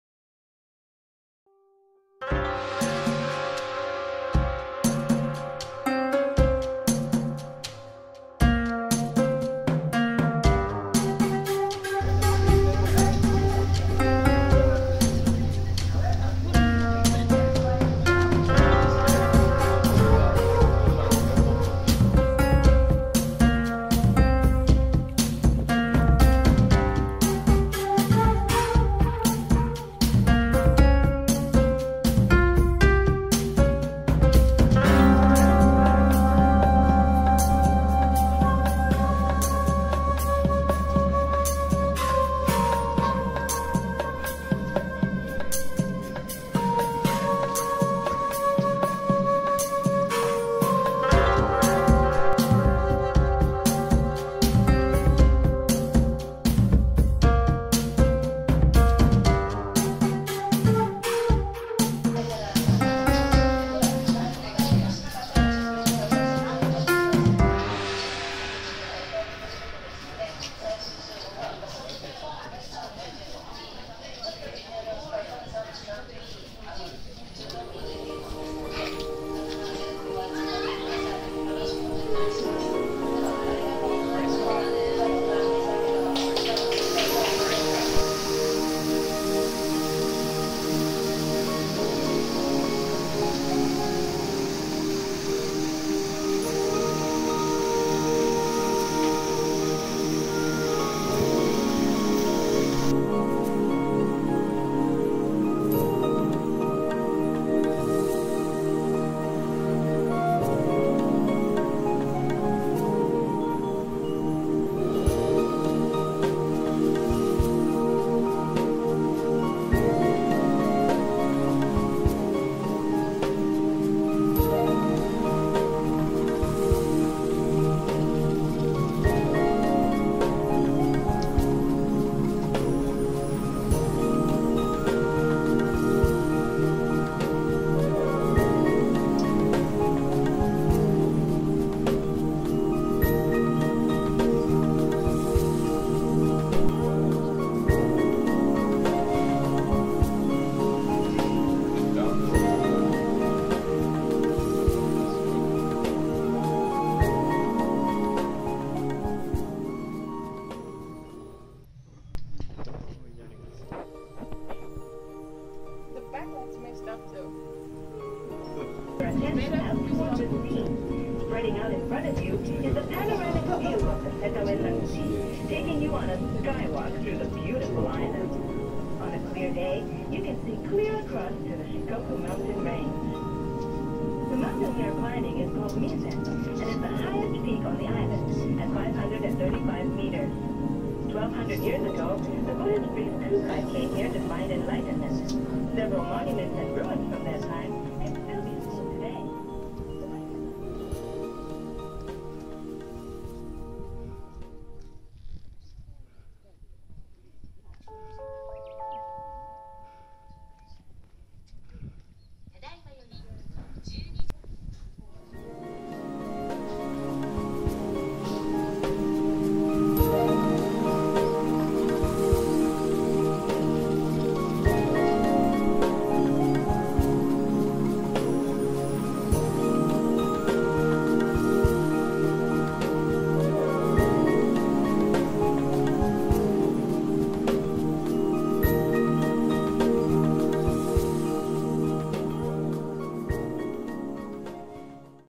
CHERRY BONSAI & natural beauty of Miyajima, Japan
I spotted a gorgeous cherry bonsai in full fruit in Miyajima, Japan. I also captured some of the natural beauty of the island and cultural heritage site, including a waterfall & views of the Misen Primeval Forest from the cableway. Full length video of our day on the island is coming soon. Check out this video of me getting attacked by deer Watch till the end to see how you can avoid my fate, lol